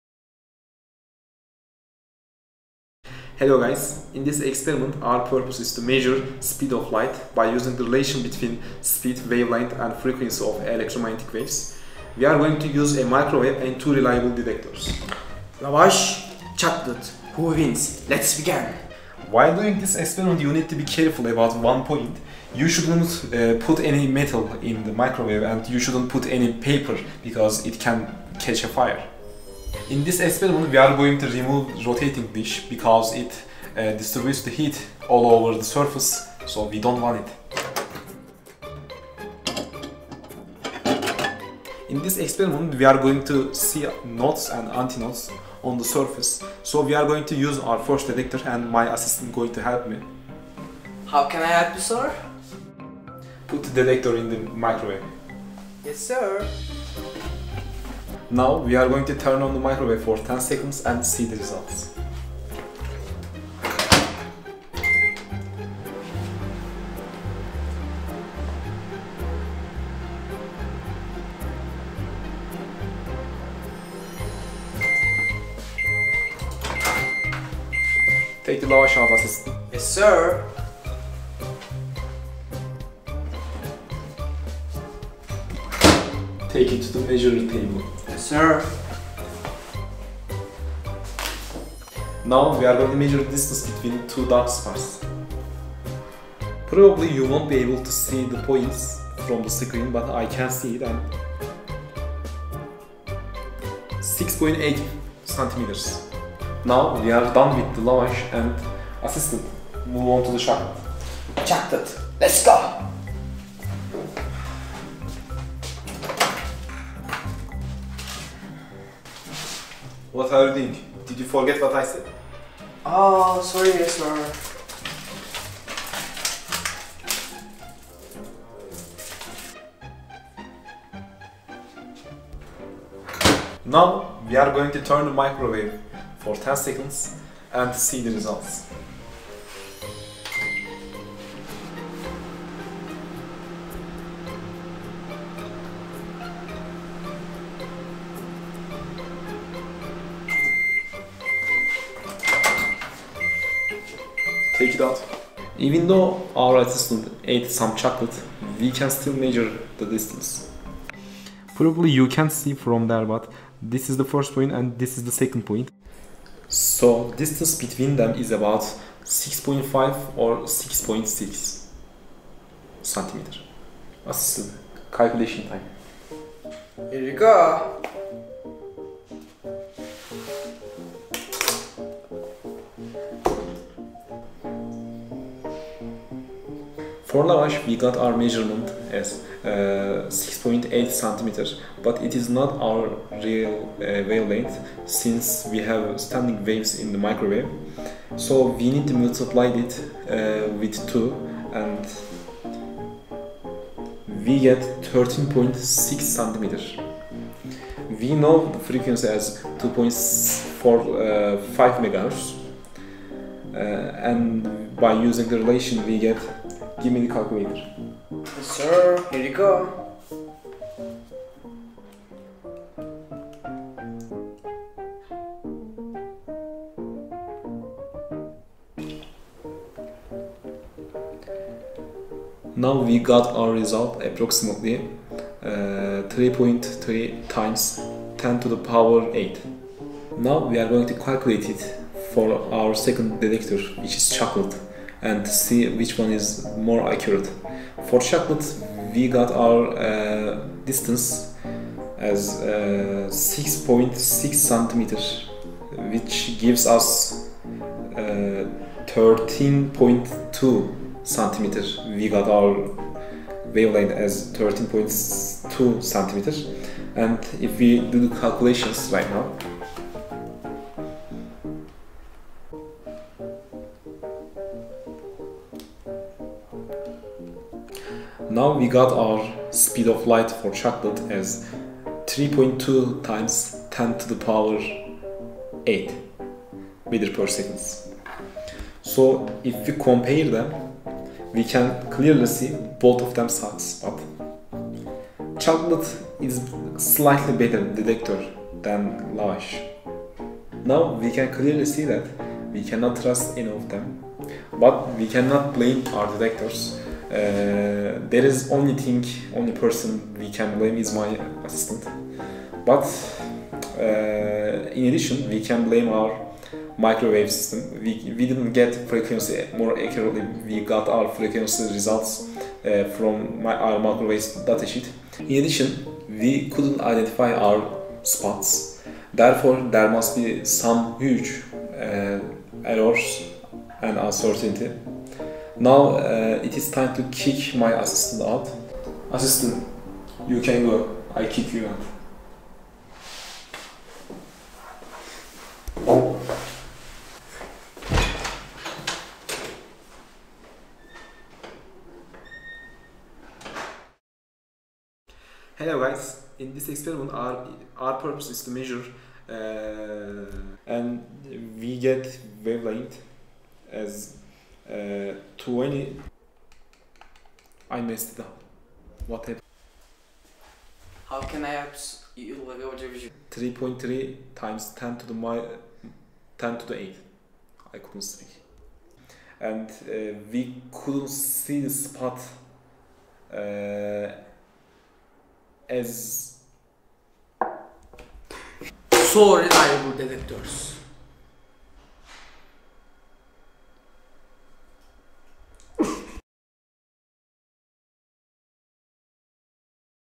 Measuring the Speed of Light
Arda and Salih measures the speed of light using simple household items.